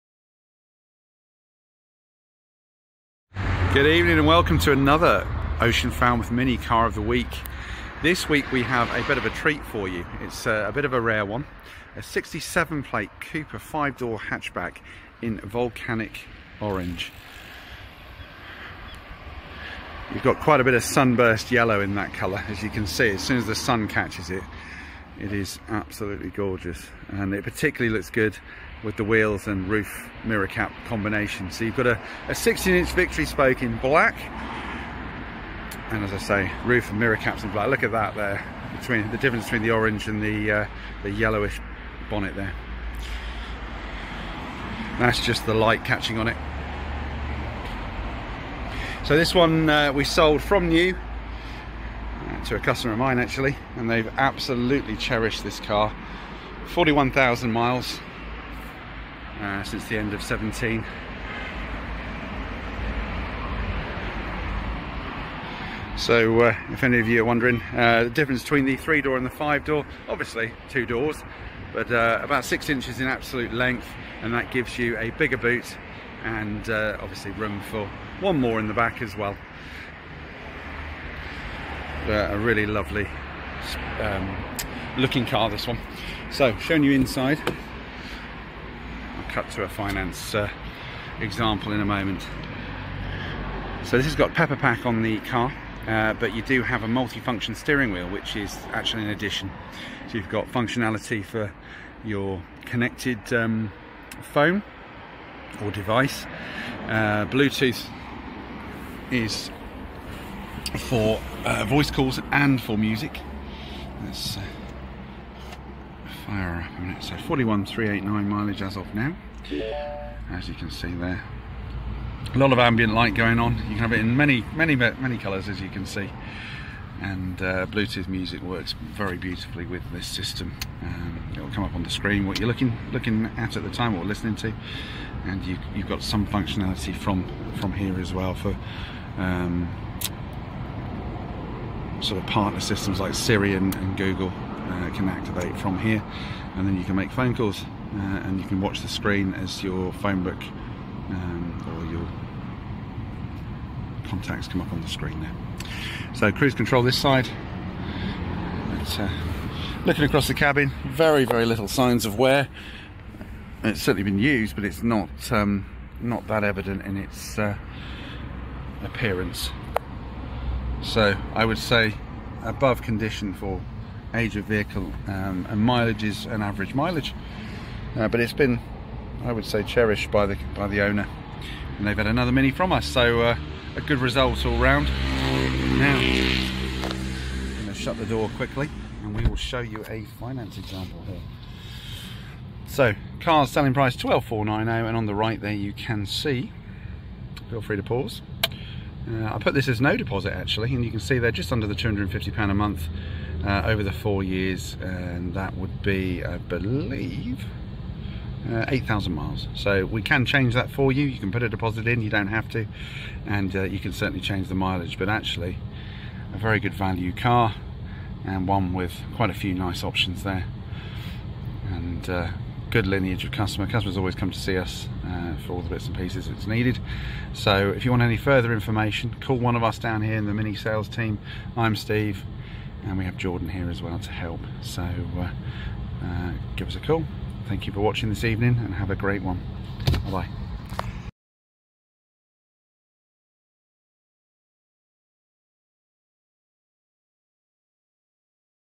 Rare MINI Cooper 5-Door Hatch | Ocean MINI Falmouth
Check out this rare MINI Cooper 5-Door Hatch now available at Ocean Falmouth. Boasting impressive features, this car has been exceptionally looked after and is ready for its new home. Watch as Steve shows you around.